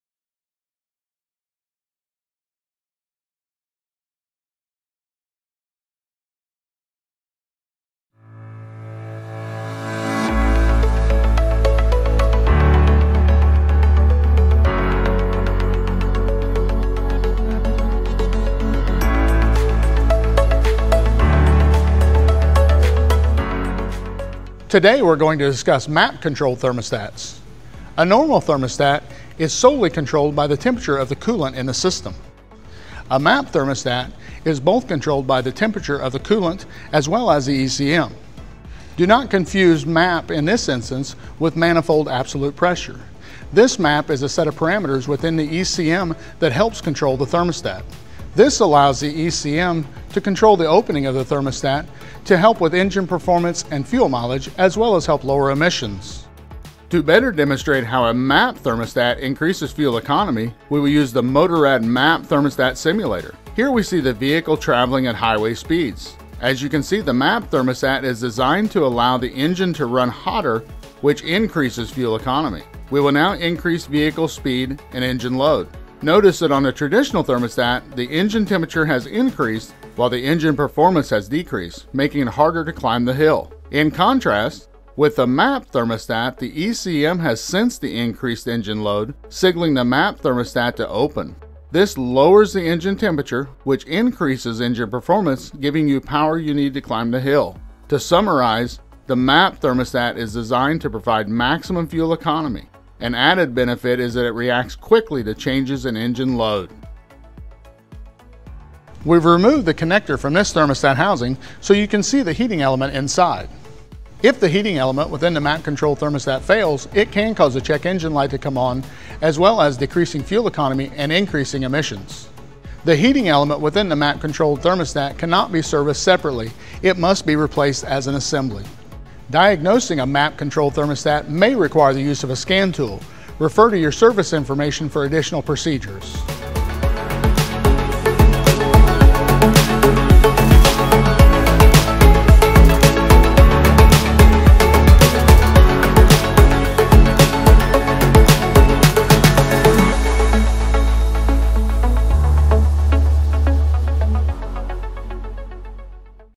MotoRad MAP Controlled Integrated Housing Thermostats
This video explains how a MAP controlled integrated housing thermostat works, as well as explains the key features and benefits that set it apart from all other automotive thermostats.